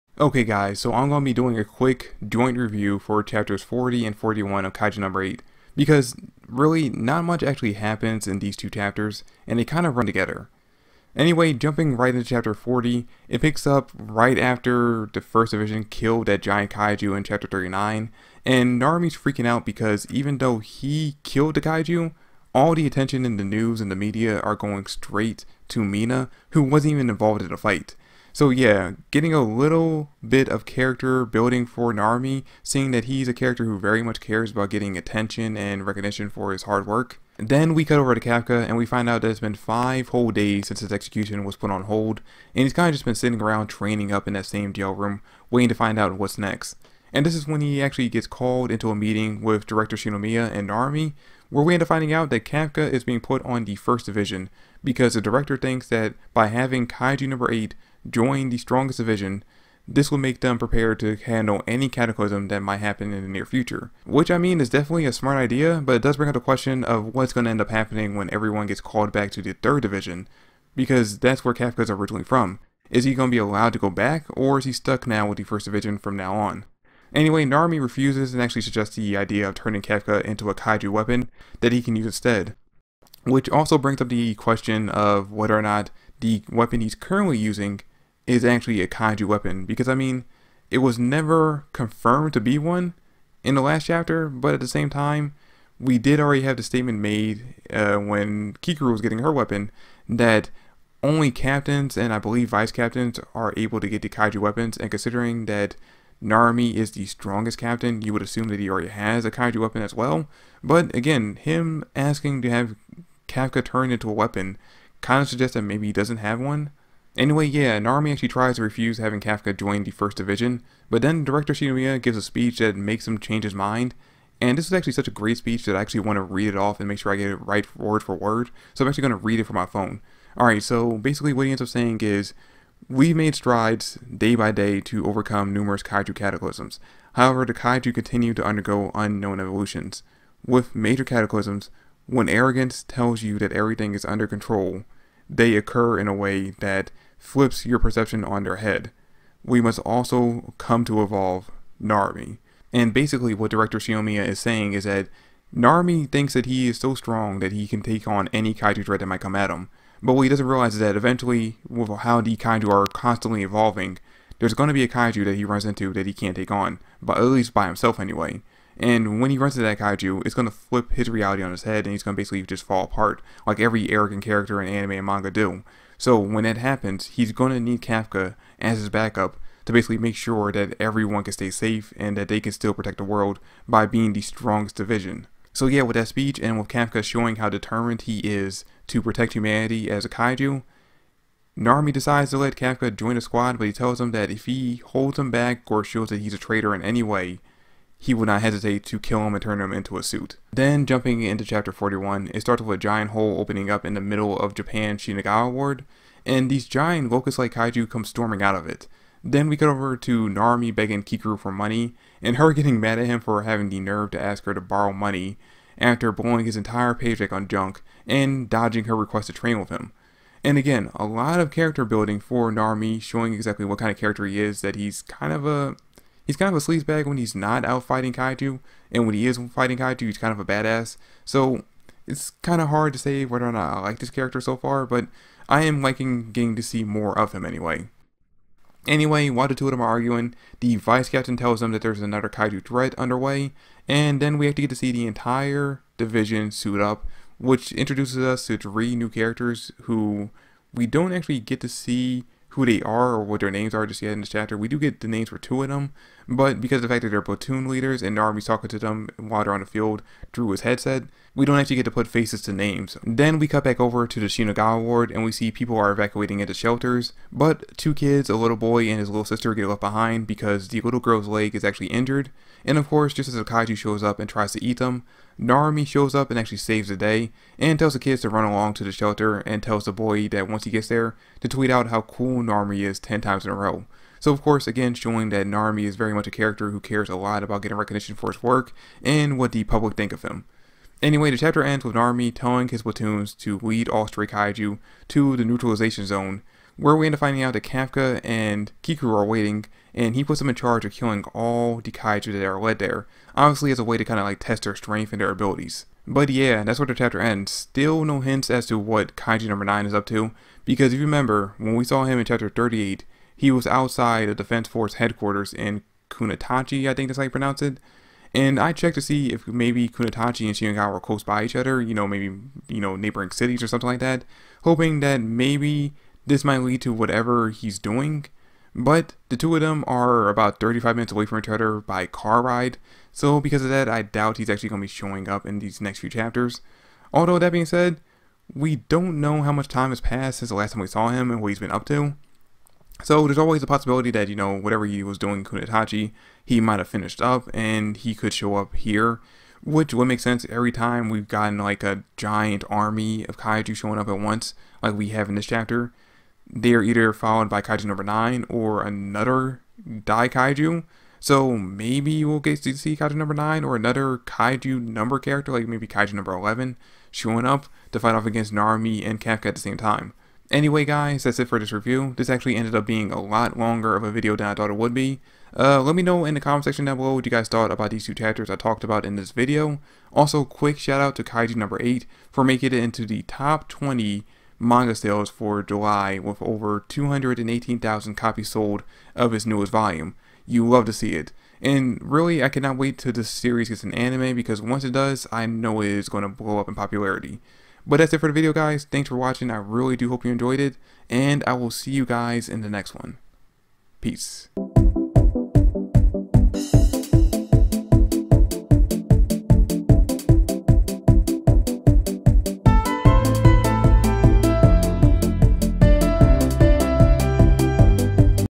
Kafka Joins The First Division -- Kaiju NO 8 Chapter 40 & 41 Review
If you enjoyed this video check out some of my other videos on this channel.

Series I Currently cover
Black Clover
Edens Zero
Fairy tail
Kaiju No. 8